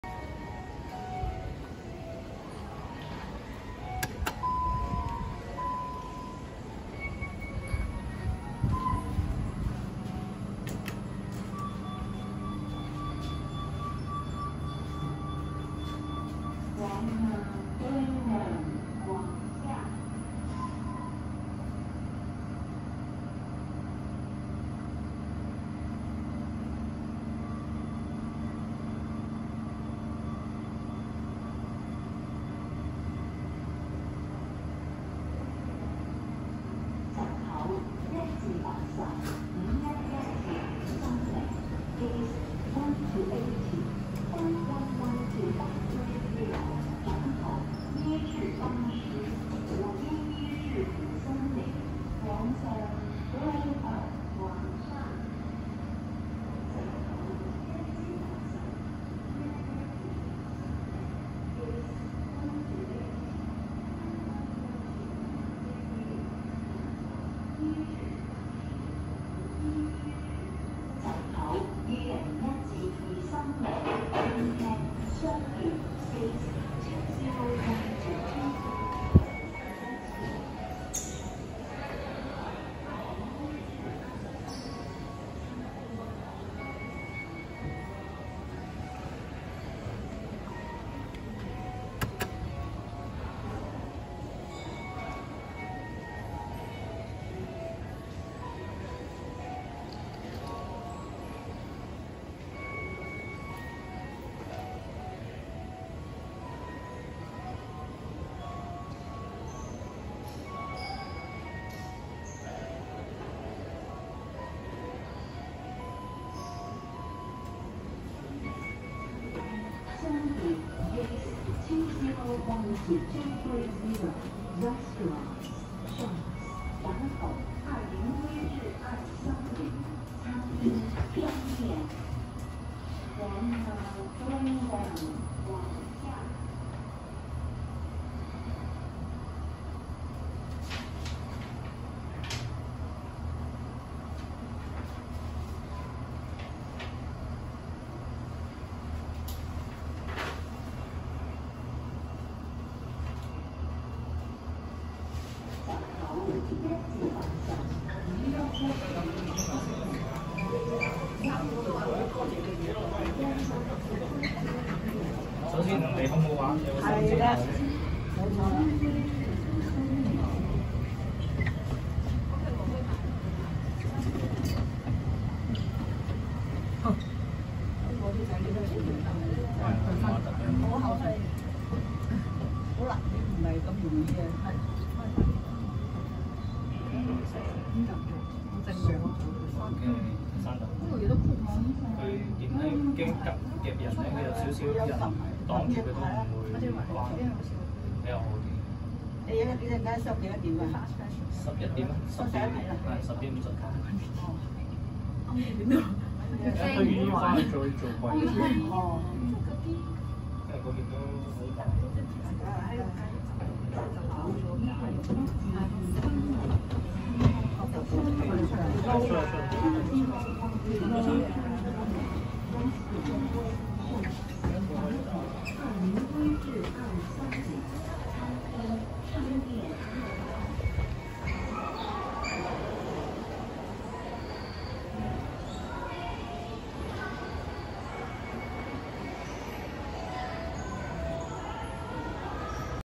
ThyssenKrupp Glass Elevators 131/132 at Hong Kong International Airport T1 Midfield Concourse
Date Filmed: 08/10/24
Manufacturer: ThyssenKrupp
Capacity: lbs / kg
Floors Served: 1, 5, 6 (6 Floors)

Floor 6: Gates 201-230, Restaurants, Shops
Floor 5: Staff Only
Floor 1: Gates 1-80, 511-530

Restricted Access: These elevators require a security credential and/or you must pass a security checkpoint before accessing these elevators.